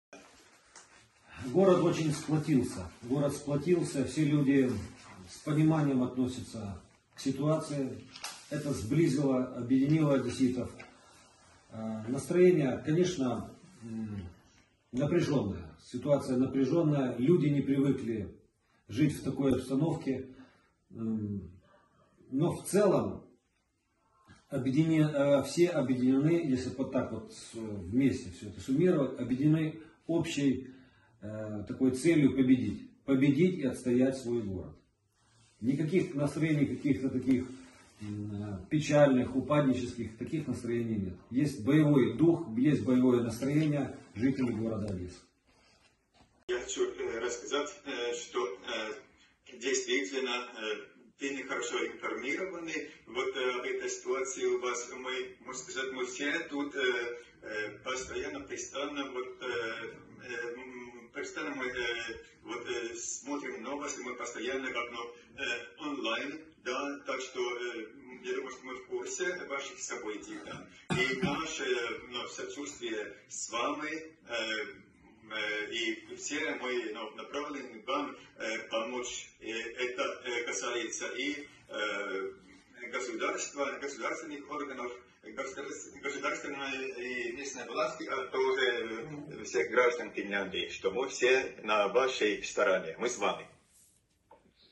Онлайн-бесіда з мером міста-побратима Оулу (Фінляндія)
Мер Одеси Геннадій Труханов в онлайн-бесіді з мером міста-побратима Оулу (Фінляндія) Пайві Лааяла:

«Одеса дуже згуртувалась, всі одесити з розумінням ставляться до ситуації. Це зблизило і поєднало нас. Ситуація напружена, люди не звикли жити в такій обстановці, але всі об'єднані спільною метою – перемогти та відстояти своє місто! У мешканців Одеси бойовий дух та бойовий настрій!».
Мер Оулу Пайві Лааяла сказала, що вони готові надавати будь-яку необхідну допомогу Одесі та одеситам.

«Ми тут добре поінформовані, ми весь час дивимось новини та слідкуємо за подіями. Ми з вами! І хочемо зробити все можливе, щоб вам допомогти. Усі громадяни Фінляндії на вашому боці», - сказала пані Пайві Лааяла.
Мер Оулу зазначила, що муніципалітети та жителі Фінляндії вже зібрали фінансову допомогу для України, яка буде відправлена ​​через Червоний Хрест.